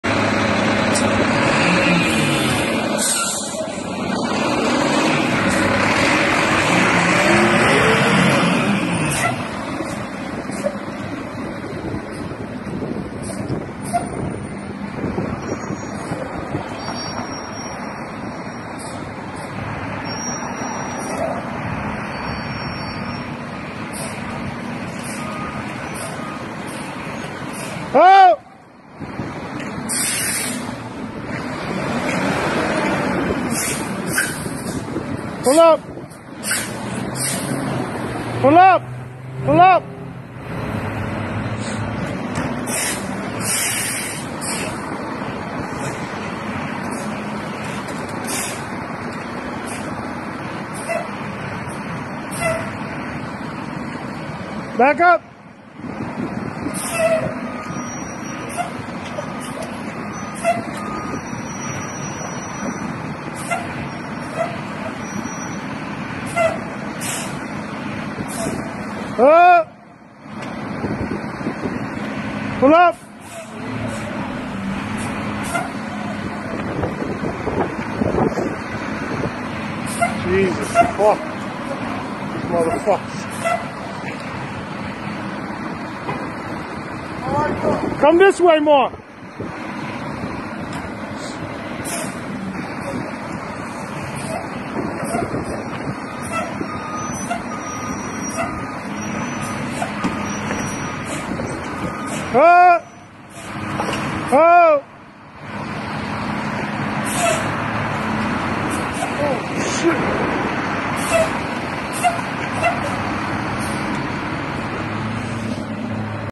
CIUDAD DE RATAS //INCREÍBLE
Como de costumbre los camiones que recogen la basura procedían a hacer su labor cuando de repente comenzaron a salir corriendo decenas de ratas que se estaban alimentando de las fundas de basura amontonadas en la calle